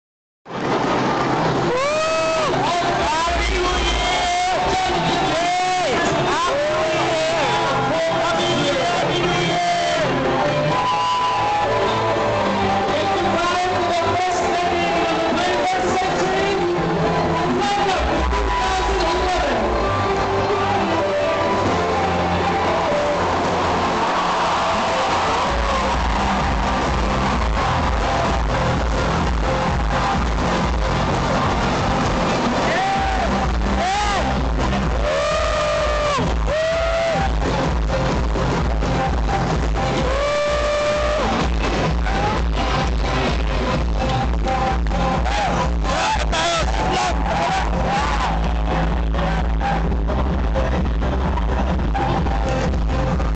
Welcome 2011!!!!
The 2011 Countdown in Eastwood with Andrei Felix =) Thanks, Drei! =)